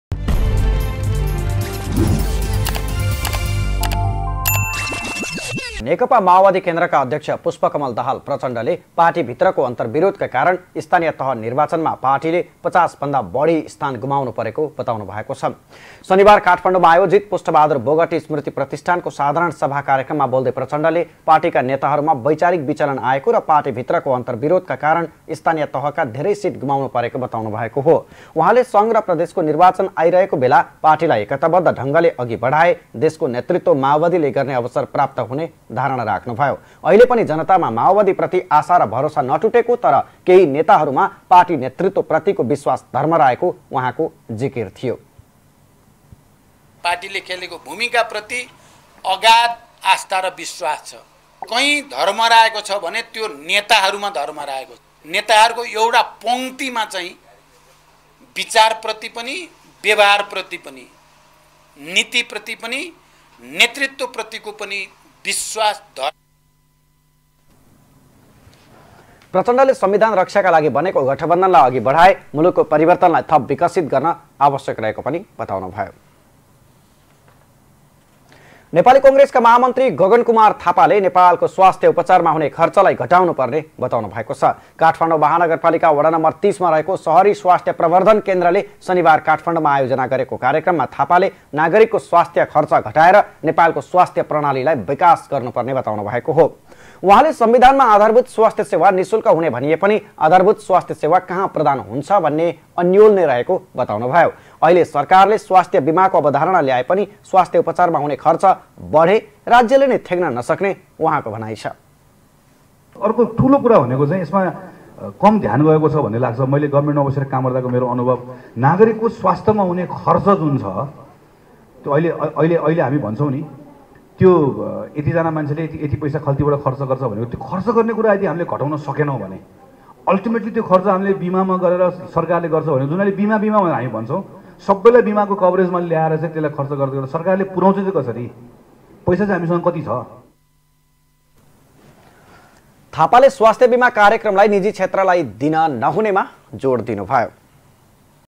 पार्टीभित्रको अन्तरविरोधका कारण स्थानिय निर्वाचनमा ५० भन्दा बढि स्थान गुमाउनु पर्‍यो :प्रचण्ड
स्वास्थ्य उपचारमा हुने खर्चलाई घटाउनुपर्छ : गगत थापा
For more videos SUBSCRICE our Channel